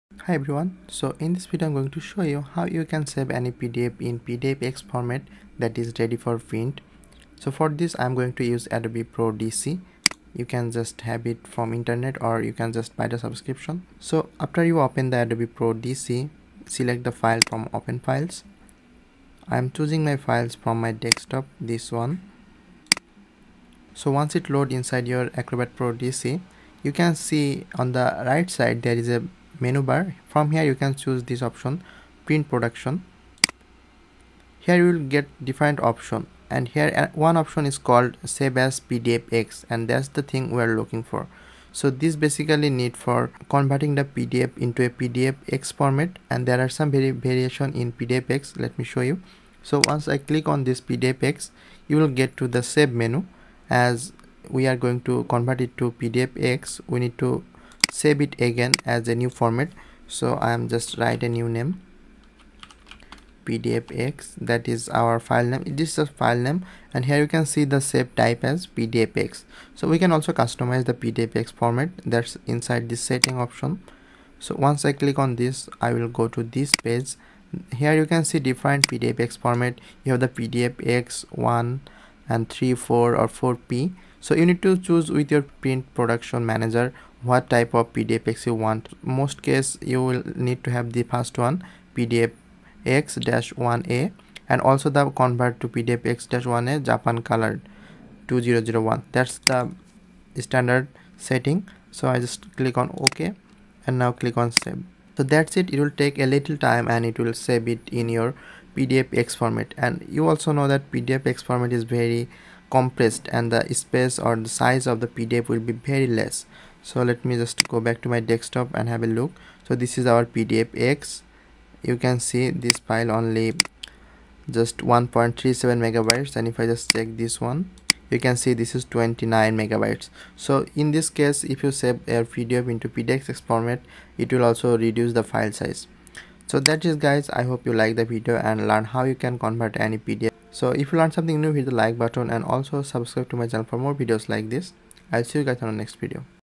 How to save a PDF as PDF-X Format
In this video i will show how to save any pdf to pdf x format in adobe acrobat pro dc.